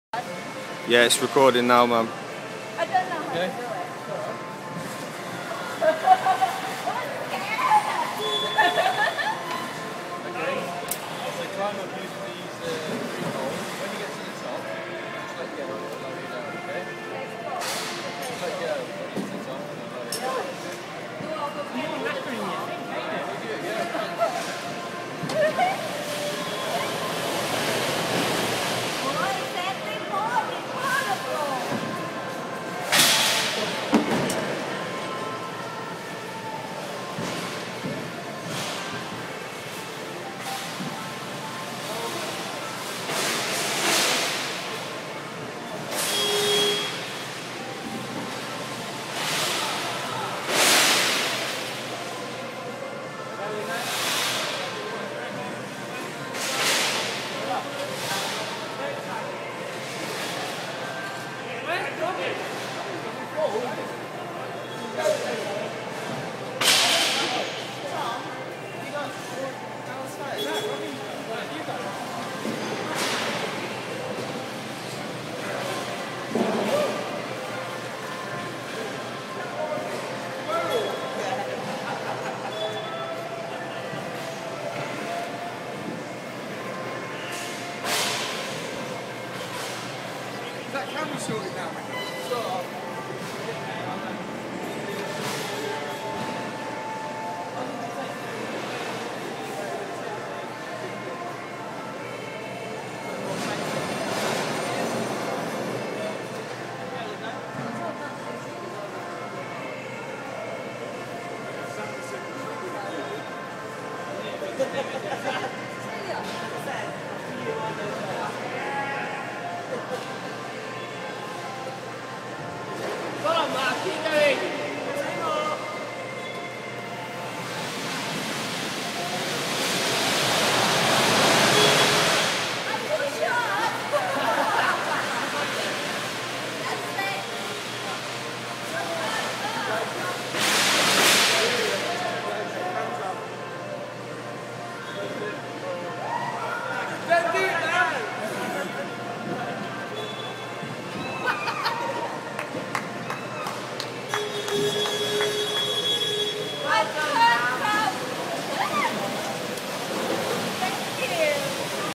My first time to climb on the wall!! ;)))
;)))) my first time to climb on the wall. I was very excited but try to be cool hehe I nearly get to on the top but mmmm I couldn't reach mmm I have to give up ;p but I did enjoy it and would love to do it again ;)))